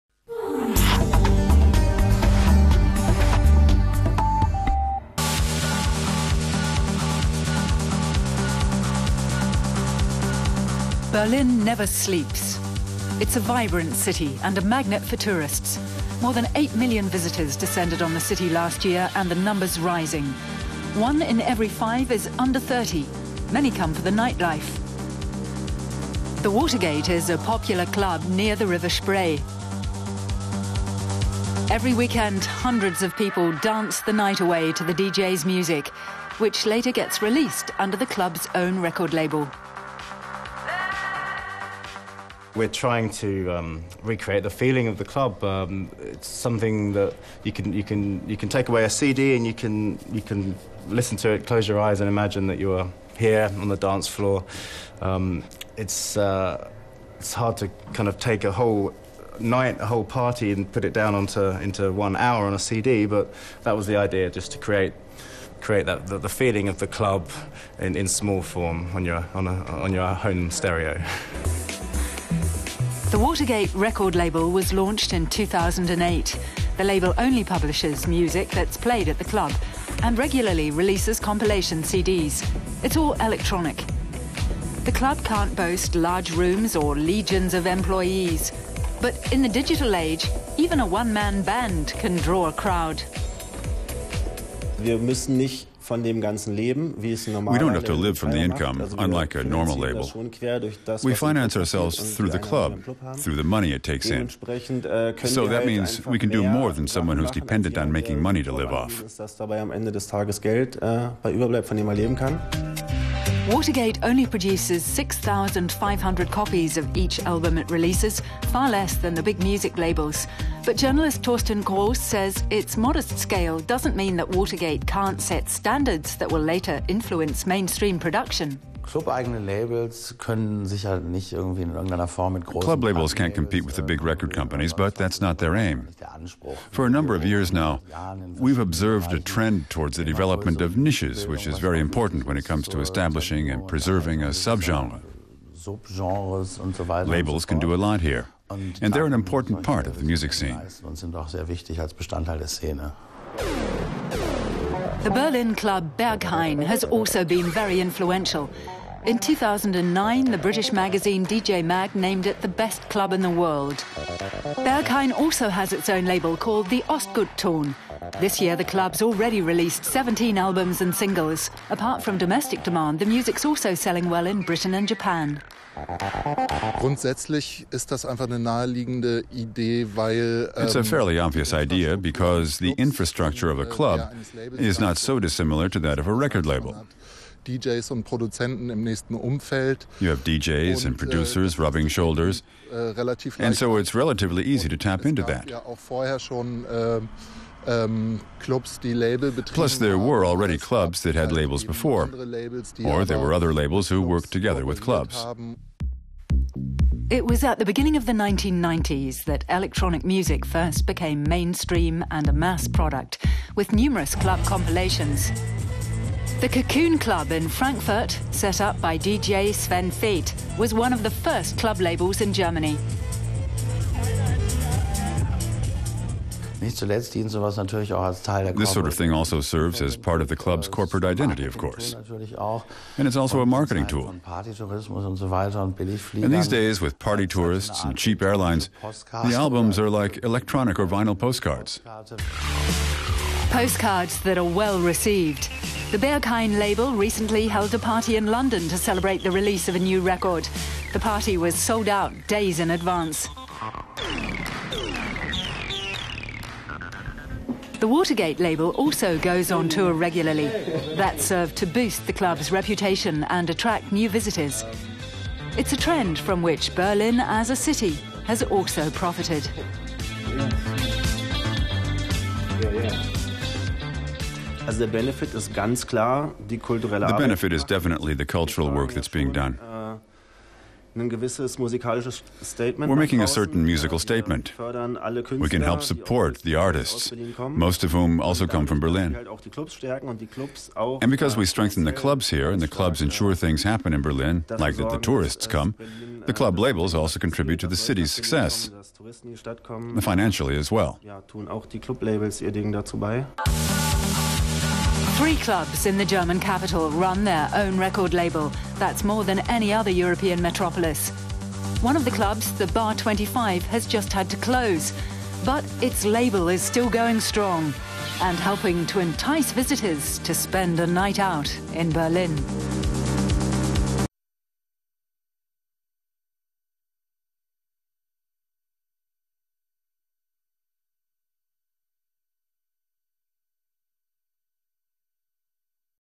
Clubbing music to take home | Video of the day
Berlin's nightlife is legendary, and the trendiest clubs have a reputation extending far beyond the German capital. Now some of the biggest hotspots like Berghain and Watergate have set up their own record labels as a way of spreading their allure even farther.